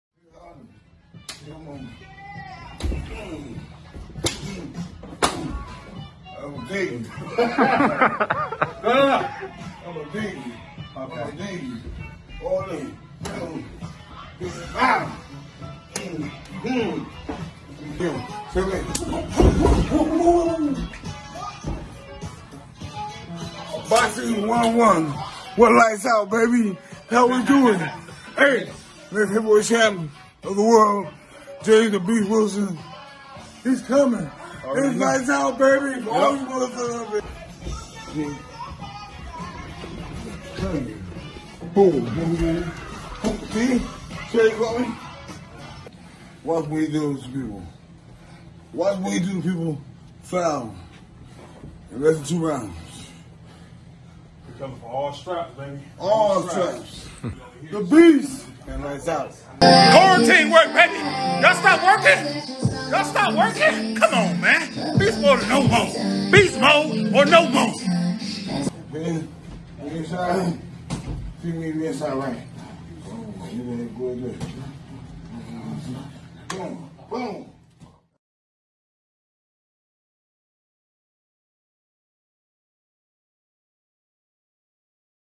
JAMES TONEY SLAPS "BOXING 101" SENSE INTO JAMES WILSON; TEACHES SNEAKY "LIGHTS OUT" TRICKS
Heavyweight contender James "The Beast" Wilson recently took to social media to post video of himself getting "Boxing 101" tips from the legendary James "Lights Out" Toney as the boxing world is on hold due to the coronavirus pandemic. You don't want to miss how they looked. Check it out!